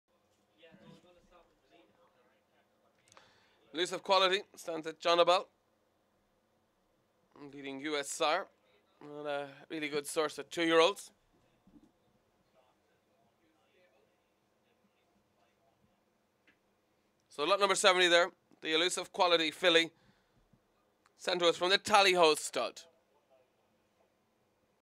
FOR SALE: 2014 Filly by Elusive Quality Ex Kenza - Breeze at Goresbridge
FOR SALE: A lovely big, strong 2yo filly by Darley stallion Elusive Quality. See racehorsetrader.com for full advert